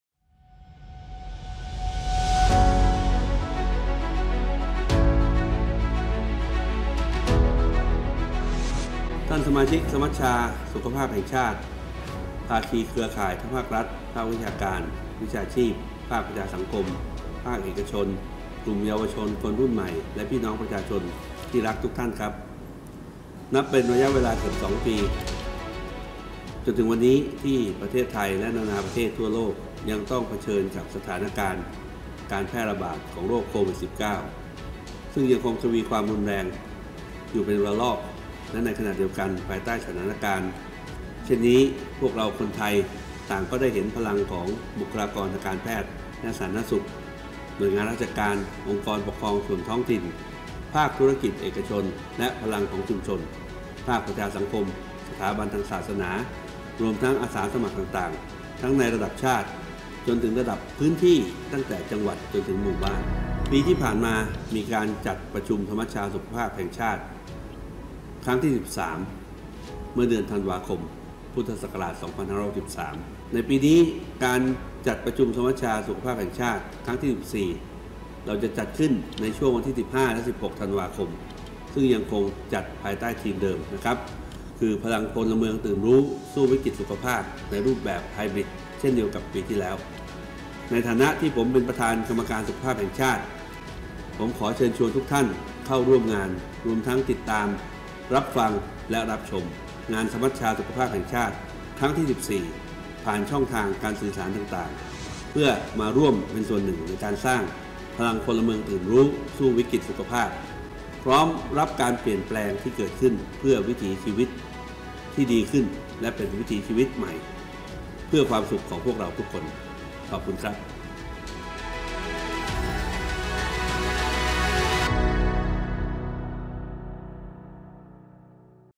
ขอเชิญชวนร่วมงาน สมัชชาสุขภาพแห่งชาติ ครั้งที่ 14
Kick Off งานสมัชชาสุขภาพแห่งชาติ ครั้งที่ 14 พ.ศ. 2564
ภายใต้ประเด็นหลัก “พลังพลเมืองตื่นรู้ ... สู้วิกฤติสุขภาพ”
โดย นายอนุทิน  ชาญวีรกูล
รองนายกรัฐมนตรีและรัฐมนตรีว่าการกระทรวงสาธารณสุข